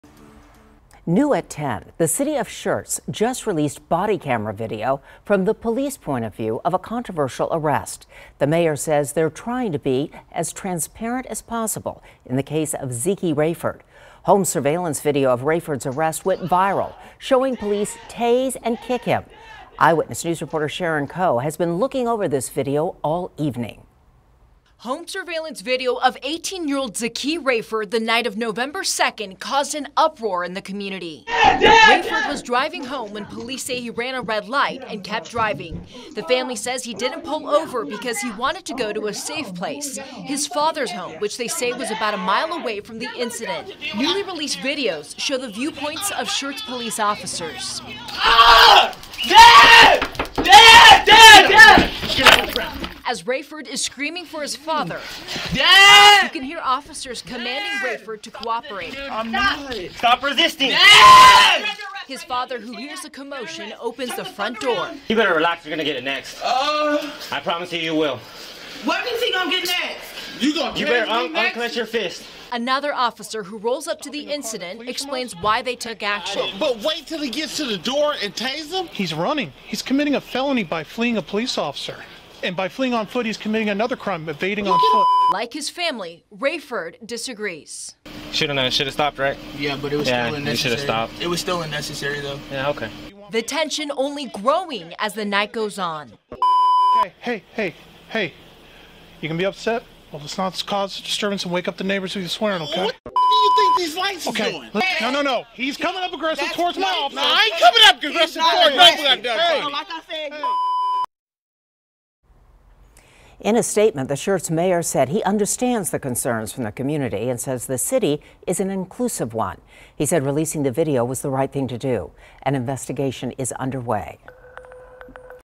Bodycam video shows Schertz police perspective, teen tased and kicked in arrest
Body cam videos are released in the arrest of 18-year-old Zekee Rayford. He was kicked and tased after he allegedly ran a red light and did not pull over.